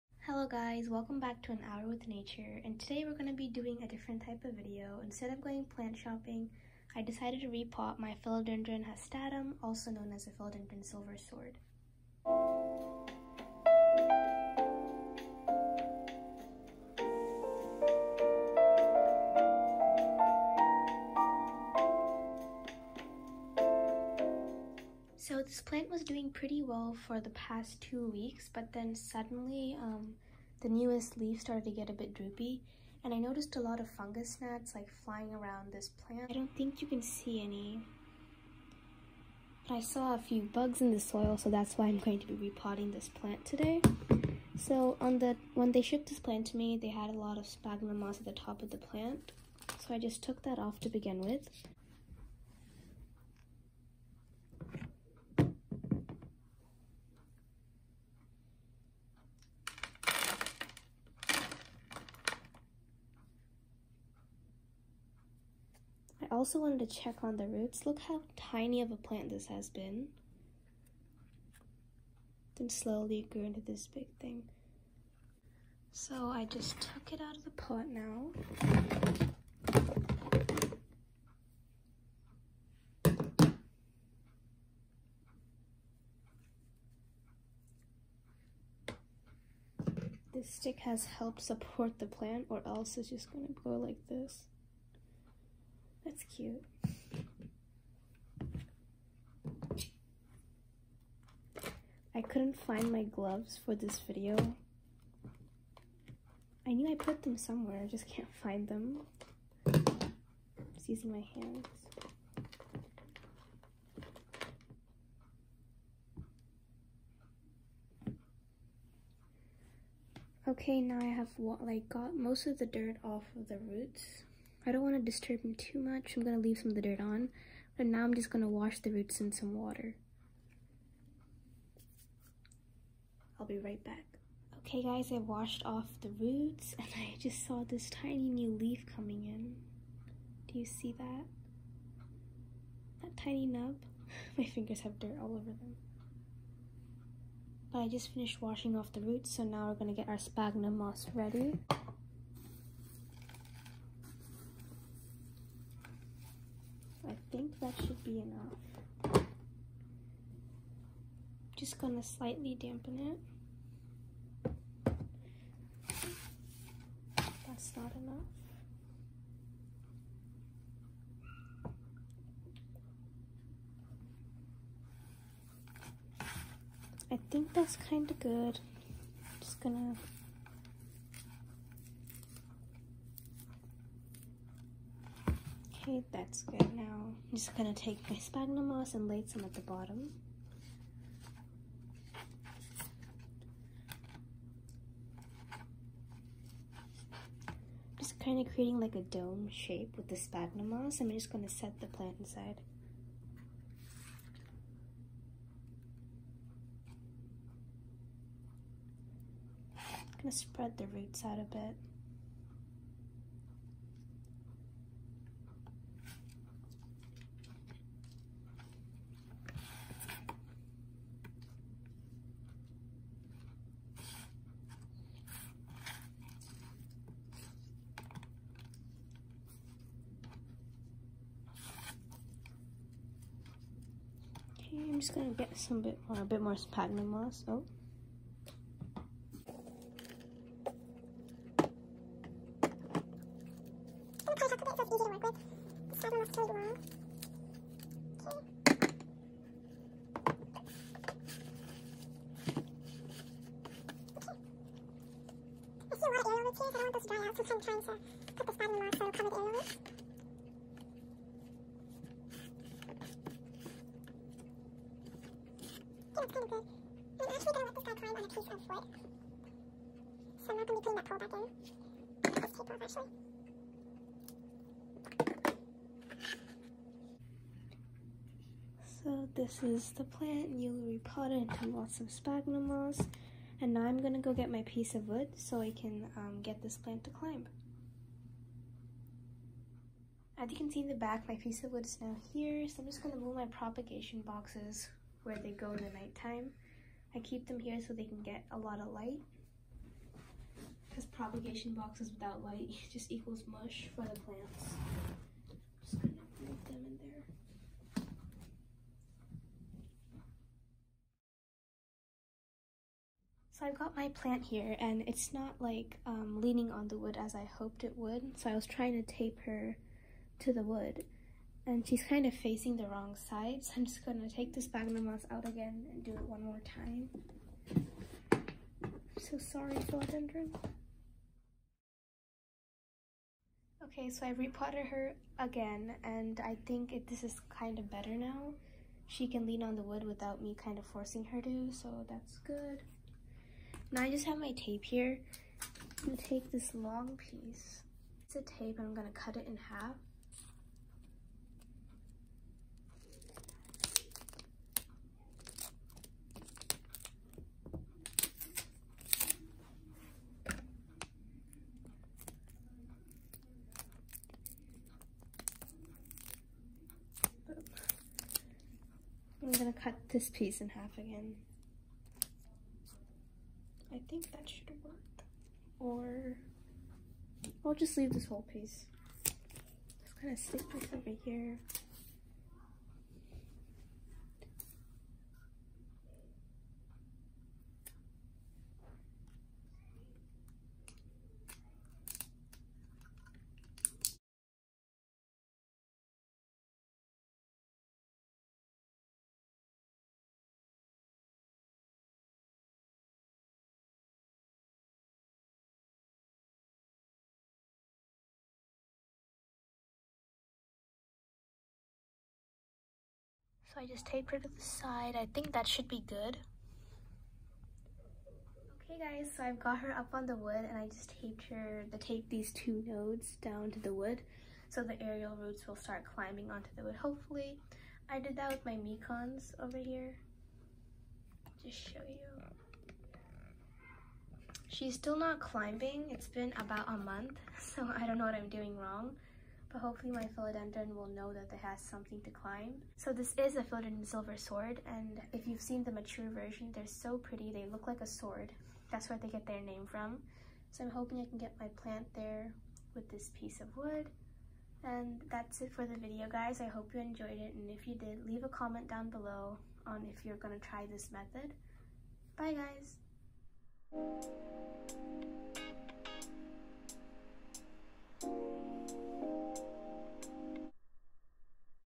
Staking up my Philodendron Silver sword
Hope you guys found this video useful! The philodendron you saw in this video is currently putting out another new leaf!
My Etsy shop